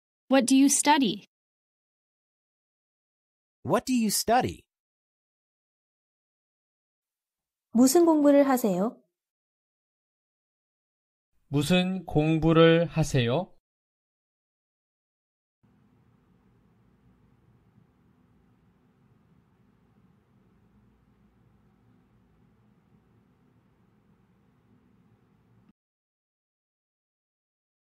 영어로 "무슨 공부를 하세요?" 말하는 법
남자 목소리 :
What do you study?

남성 음성 번역 :
무슨 공부를 하세요?